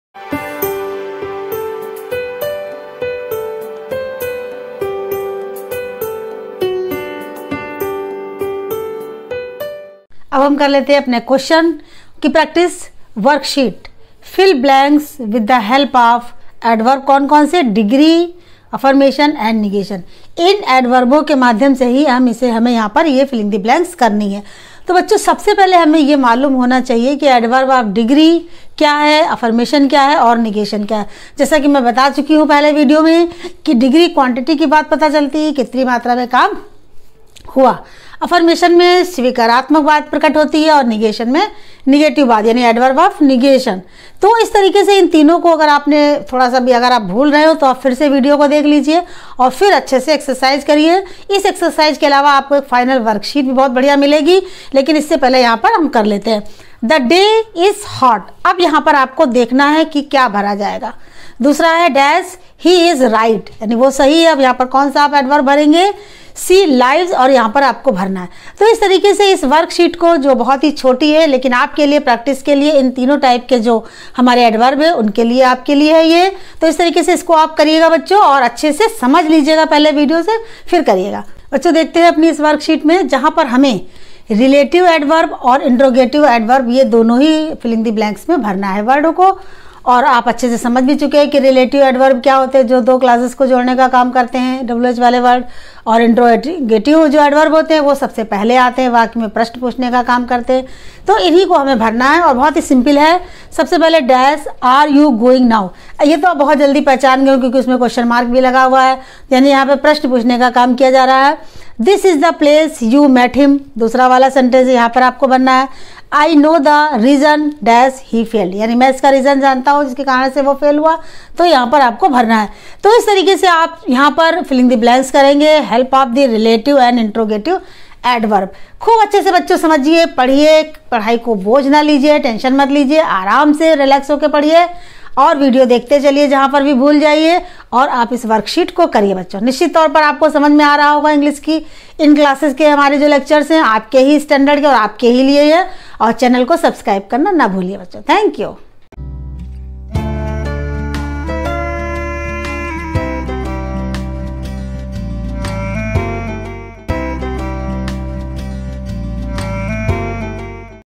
Adverb | Parts of Speech | Question Practice | Part - 01
Hello Everyone
In this video I will be discussing the Question Practice of "ADVERB". I hope you all will like this video.

Sunita Srivastava